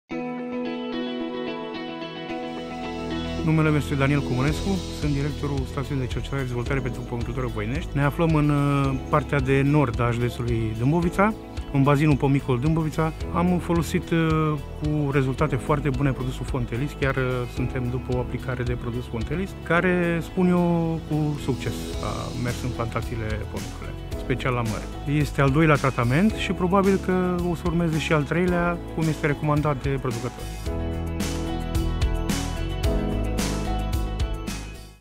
Testimonial Fontelis® - Daniel Comănescu
Fontelis® este un fungicid sistemic pentru combaterea bolilor foliare la măr.